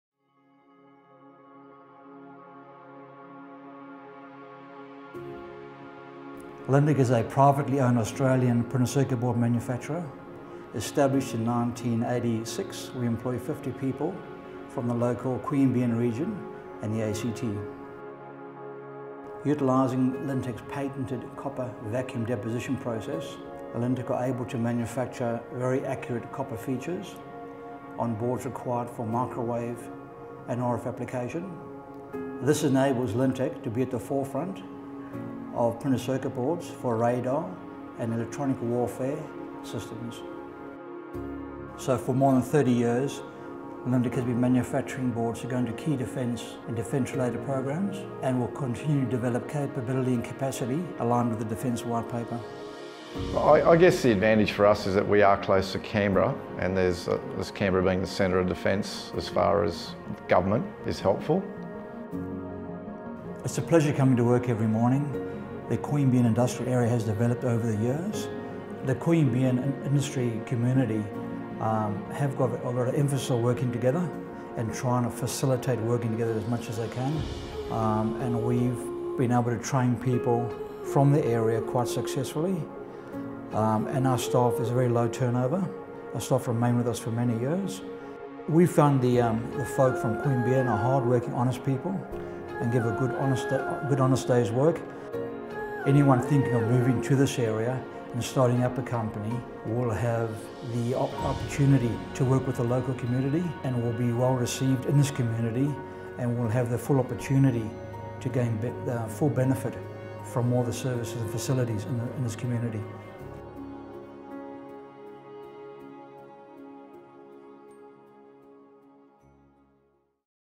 Lintek - Canberra Region
Established in 1986, Lintek is a manufacturer of Printed Circuit Boards (PCBs). The company supplies its products to government departments in Canberra, commercial businesses nationally, and select overseas markets.

Lintek’s Managing Director, Carl Tengstrom, says that Lintek’s patented copper vacuum deposition process enables the company to make very accurate copper features for PCBs that are required for RF and microwave application – which is particularly important for radar application and electronic warfare systems used for defence.

Technical Director, Renato Morosin, says that Commonwealth support and funding has allowed Lintek to purchase new equipment (most of which has been manufactured locally), which has been great for business and expanded their capabilities to further meet client needs.

Based in Queanbeyan, Lintek employs around 50 people from the local region. Carl says that they find the people from Queanbeyan to be very hard-working and honest, and the company benefits from a very low staff turnover.

Renato, who lives in the nearby suburb of Jerrabomberra, says that it takes very little time to get to work in the morning, and he enjoys being within five minutes of everything they need in Queanbeyan.

Carl says that anyone thinking of moving to the area to start a business will have the opportunity to work with a great local community, and enjoy the benefits of the range of services and facilities that Queanbeyan offers. “The Queanbeyan business community have a lot of interest in working together, and try to facilitate that as much as they can. It’s a pleasure coming to work every morning.”

You can find out more about visiting the Queanbeyan-Palerang region and living and working in the region